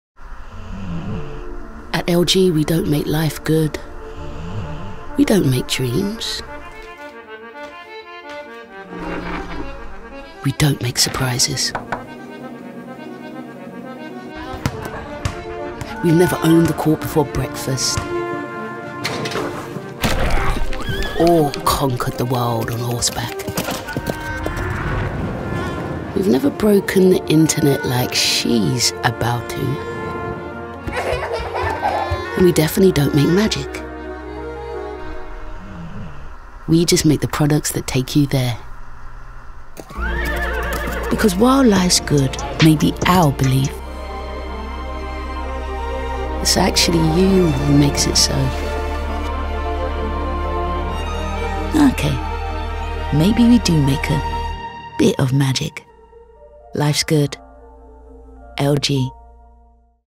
Life's Good | Nosotros no hacemos que la vida sea buena: la haces tú
En LG, no tenemos la clave para que tu vida sea buena. Sólo somos la marca que te anima a ser optimista, a seguir intentándolo pase lo que pase.

Porque aunque Life’s Good sea nuestra filosofía, eres tú quien decide hacerla realidad, por ti y por los tuyos.